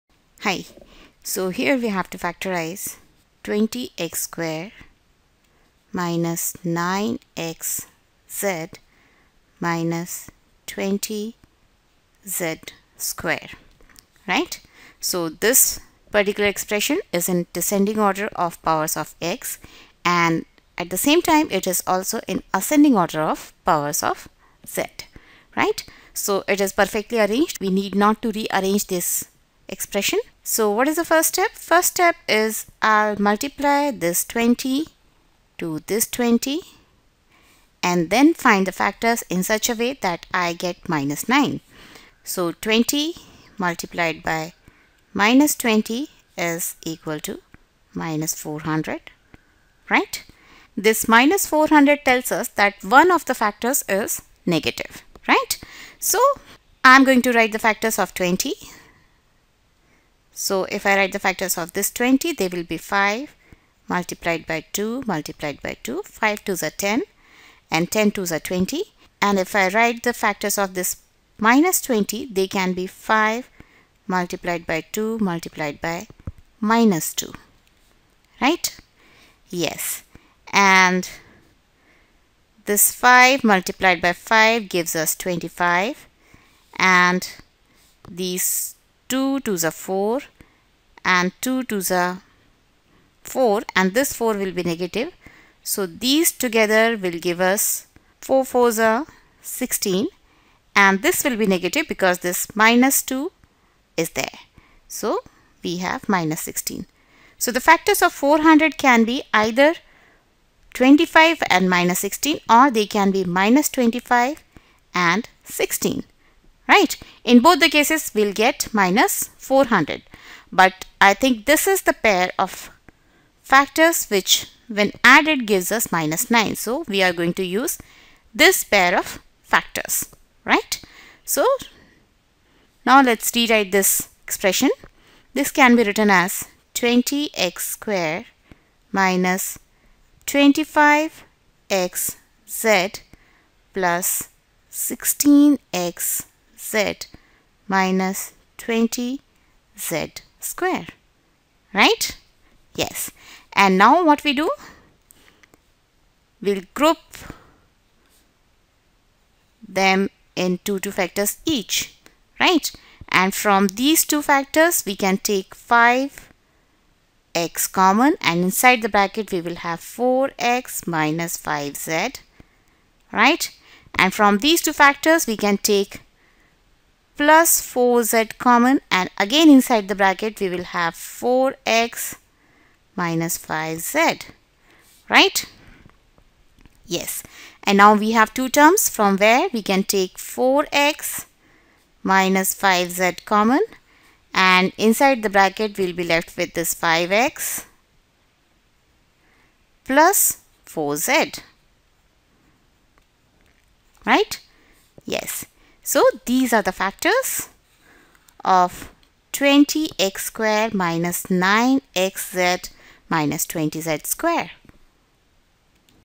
Factoring quadratic expression in two variables
Factoring quadratic expression in two variables is very similar to factoring quadratic expression in one variable. Multiply the first and last term and find the factors of the product in such a way that the sum of factors yields the middle term.